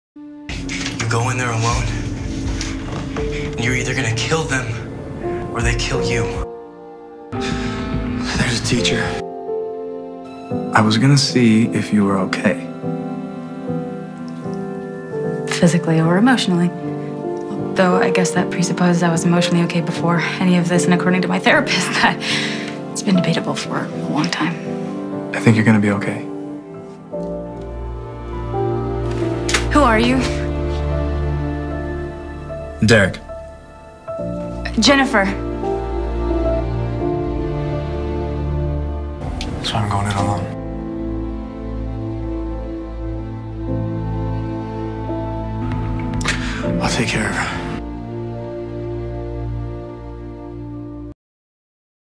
::Derek&Jennifer::
I AM STILL A STEREK SHIPPER! This is my contribution to the ship which has received so much shit that they deserve this... Honestly I like Jennifer, not with Derek, but I like her..

I like that she makes quips about her mental state because she doesn't know how to deal with her life. I like that she doesn't panic when Derek approaches her bloodied and that even if she's scared out of her mind, she puts up a front like she's okay, no matter how cracked it might be. Jennifer is a character I can relate to because I've through therapy, I've been through someone telling you that you're not stable. I've been through not wanting to talk about something because you're afraid about what people will think about you, that you've gone insane.. I don't ship Jennifer and Derek because to me that's the blind leading the blind.

STORY/SETTING?: They're two people who are broken trying to fix each other with out knowing anything about each other, with out bothering to know anything because they're so desperate for someone to cling to that they just grab on to the nearest person around them, even if that person would probably drown with them. They're two swimmers being circled in by sharks holding on to a life raft that has no guarantee of supporting them. And in the end, its obvious Derek's got more to loose. Because he blames himself for his family, Erica, Jackson, everything as is and if Jennifer dies, because she probably will, he'll blame himself more. She a liability Derek can't afford, not while the alphas are around, but he's taking the risk. And he won't let her get hurt, he can't.